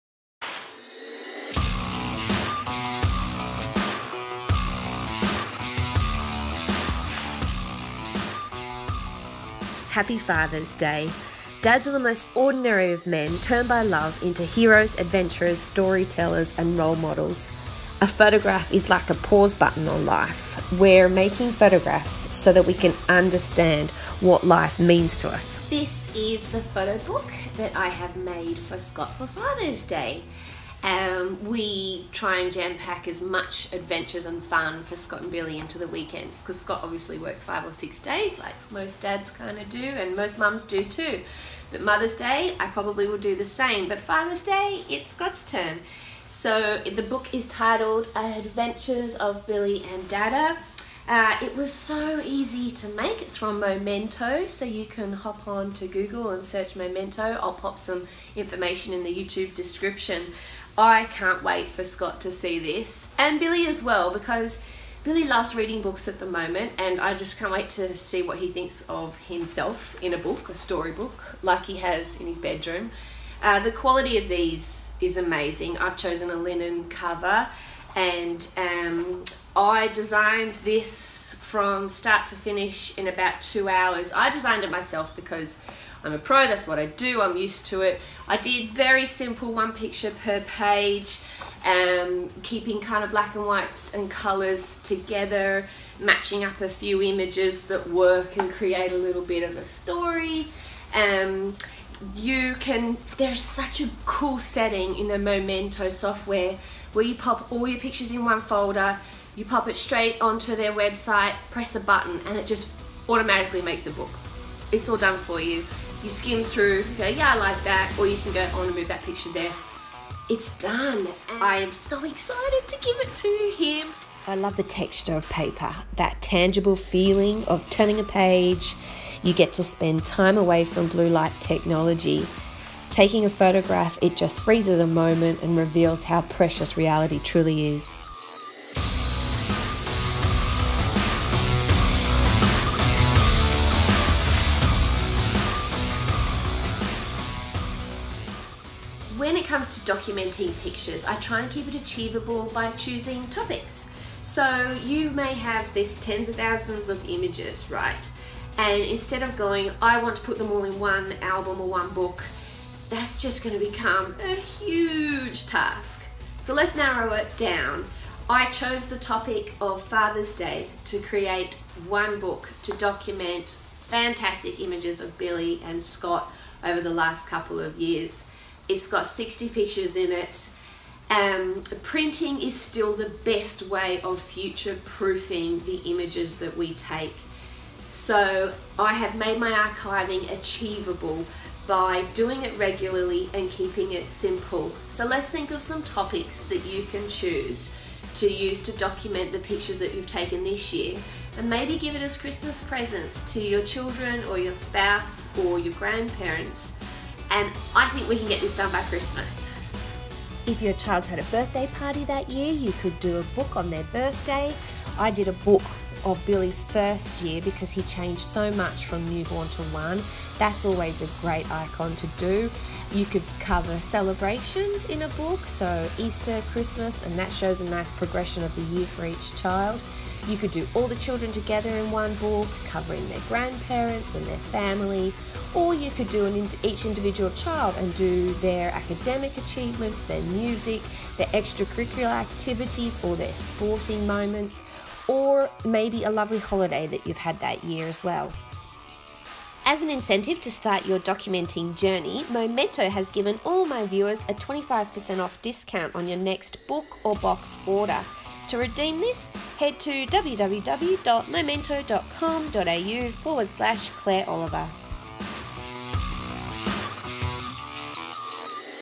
How to photograph your children: Photo Books - Father's Day Special
I hate shops/shopping. I know. weird right! So when it comes to Father's day, Birthdays, Christmas I love giving framed pictures, books or albums... so this Father's day I once again avoided the queuing at my nearest shops as I gave Scott a Momento Photo book. I filled the book with images of Scott and Billy and all their fun adventures. Took me only 2 hrs from start of editing the images, to popping some filters on and then uploading to the Momento website.

Documenting our images can be an overwhelming task if you don't do it regularly...leaving all your images for one book once every few years can be a bit daunting. so just start now and DON'T OVER THINK IT! (Once you get the hang of making one book feel how rewarding and easy it is and you will be able to follow up with old books you've been meaning to do and the task will become simpler) Start today, tackle the past tomorrow!

I try spread my documenting out into different mediums (prints, frames, books, albums).
I chose Father's Day as a great excuse to make a photo book purely of Billy and Scott. I might do one for myself for Mother's day next year. This book has dual purpose, Scott was over the moon with the it (happy husband tick!) I also got to knock over some documenting of Billy this year.
AWESOME!